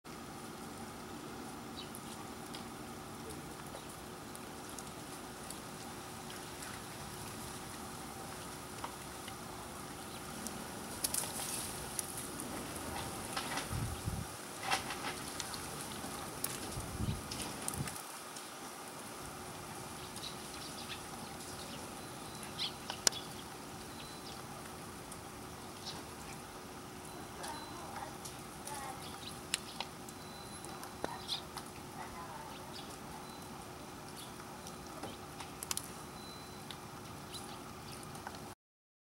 都会の雪の中のスズメ（再UP）
２０１３年１月に撮影。前回、手ブレ補正を受けた為に分かり辛くなってしまったので今回は補正無しで再アップしてみました。